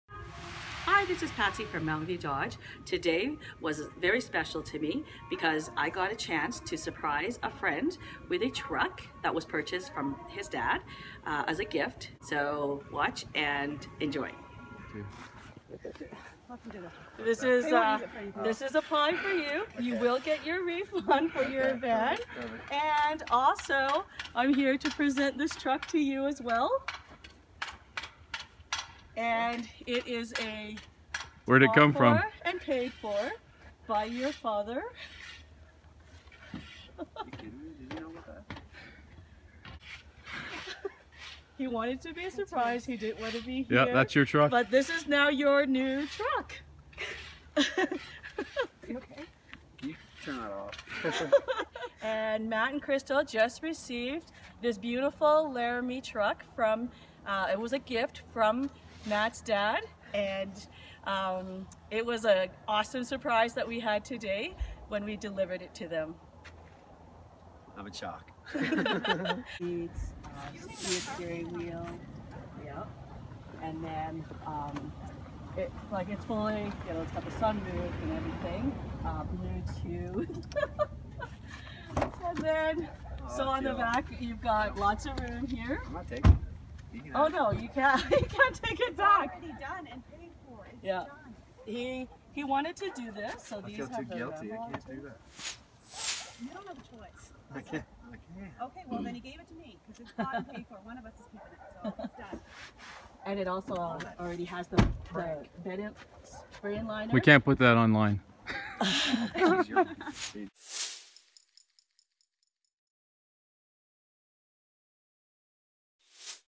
Matt Got a Huge Surprise From His Dad & Mountain View Dodge
Matt just received a huge surprise from us; Matt’s father from Nova Scotia came to visit them for the holidays, during his time here Patsy had the privilege to help him purchase a truck for his son.

Mountain View Dodge surprised Matt and Crystal by delivering the truck to their home.

We had to make up a really lame excuse to stop by after Matt’s long shift at work.

He had no idea that his father did this and it was difficult for him to accept such an elaborate gift. His father also purchased an extended warranty, the Chip, and insurance!

We hope you enjoy the 1500 Laramie! It is moments like these that selling vehicles isn’t just about transportation needs but about building relationships.

We were honored to be a part of this experience, we thank you - all our clients and are proud of being in business with such a great community!

32580 11 Range Road #43
Olds, AB
T4H 1P6